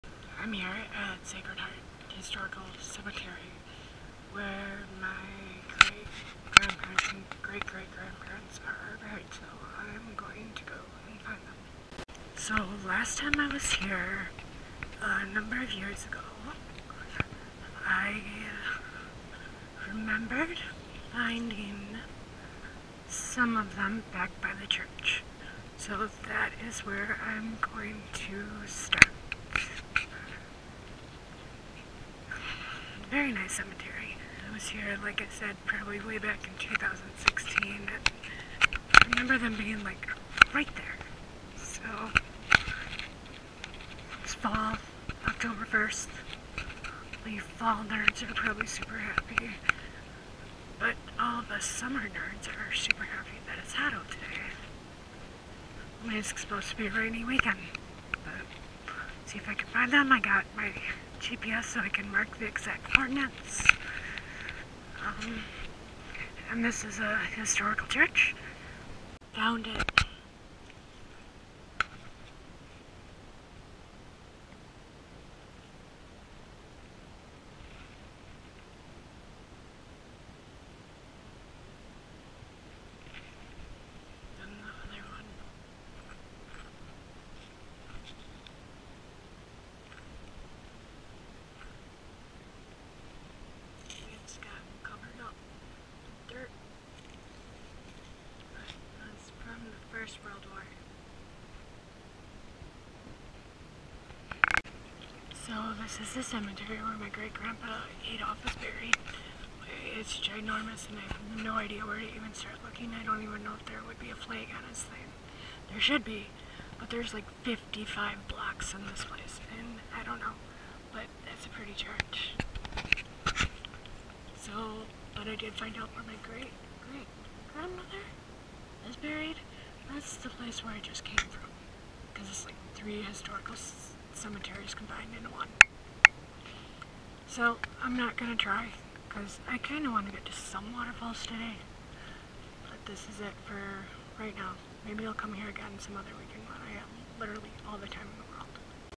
Family Graves 1
Some familial graves in Eau Claire, WI. I have more to find, but I didn't have the time available to devote to tracking some of them down.